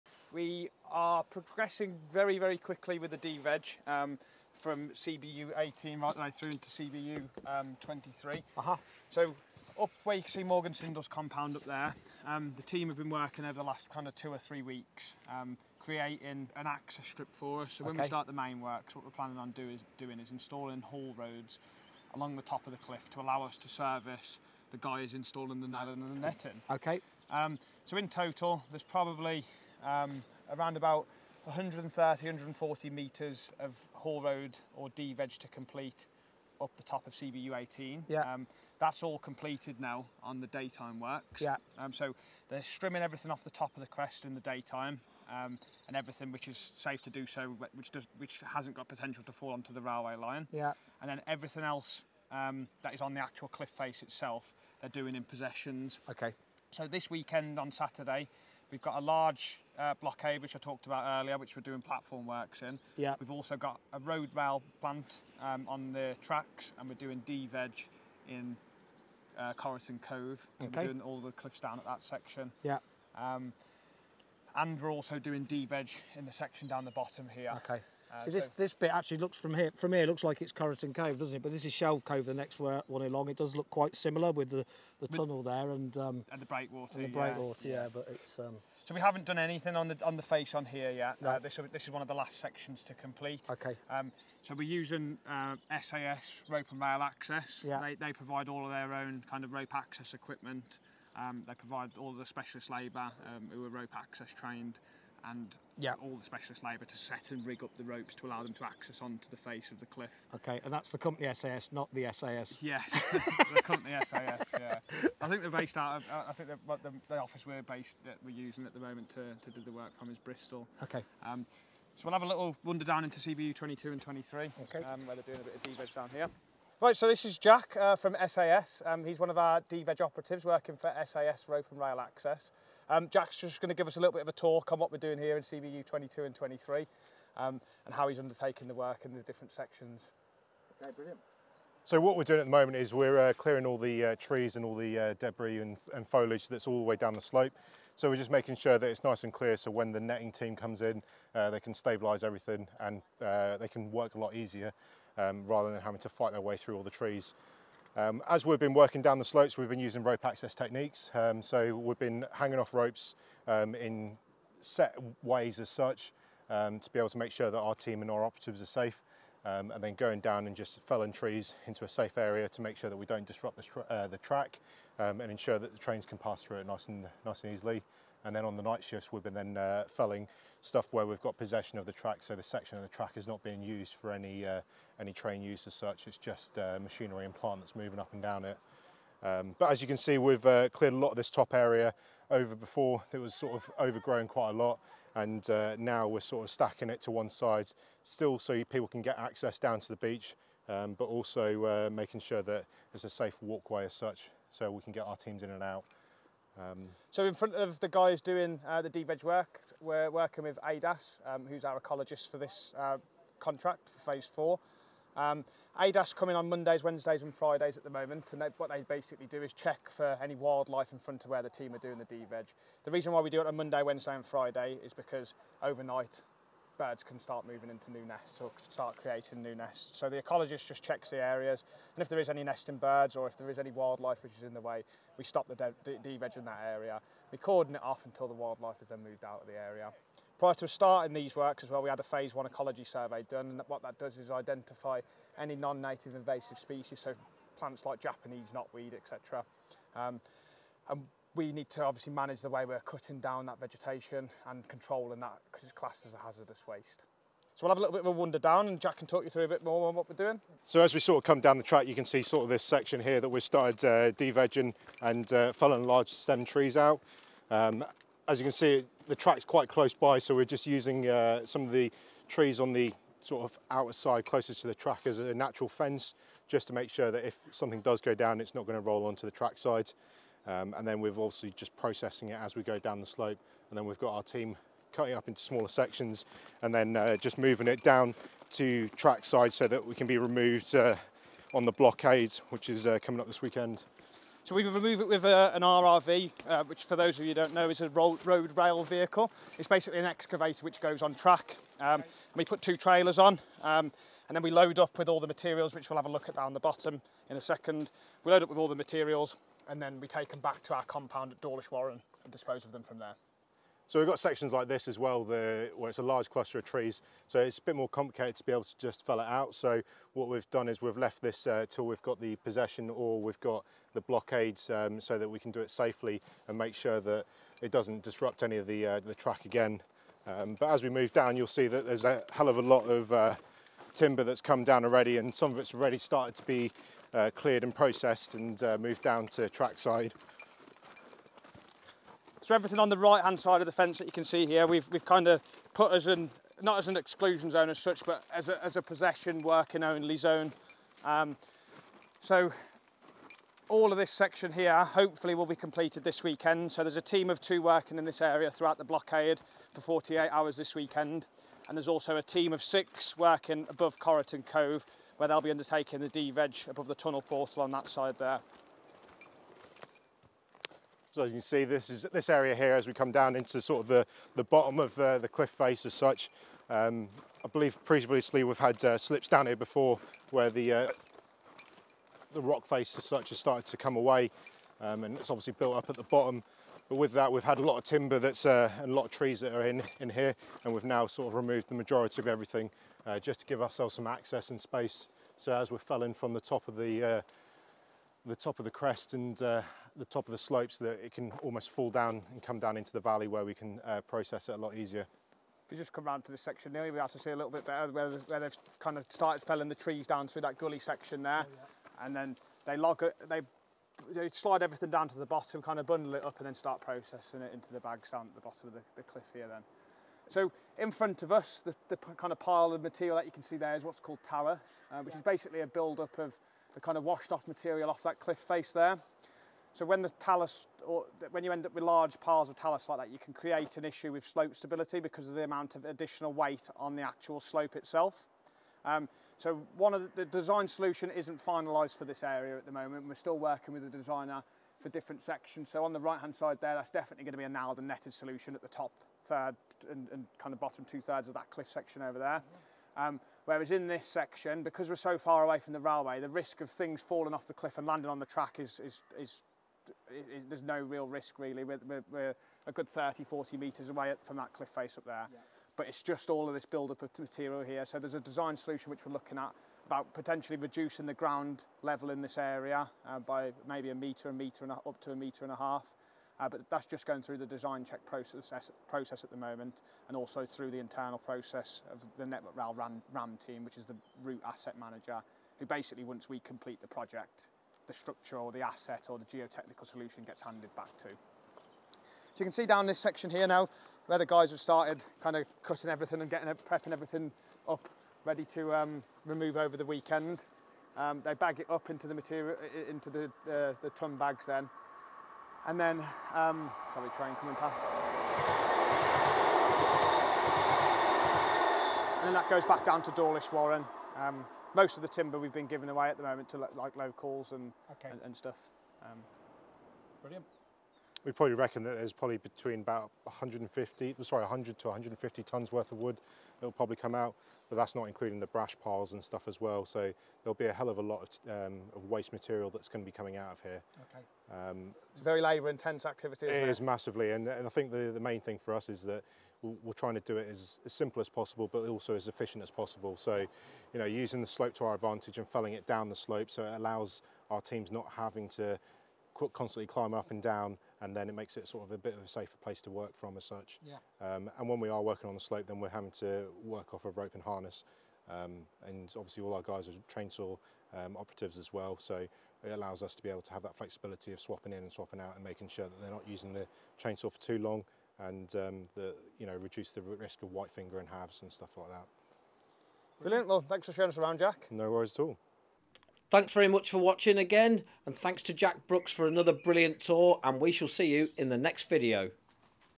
Dawlish Sea Wall Update - March 2023 Part 2
In part two of our March Dawlish sea wall update, we take a trip over to phase four with Jack Brookes from@BAMNuttallTV and Jack Wiseman from SAS Rope & Rail. They show us what they are doing in the different areas along the cliffs.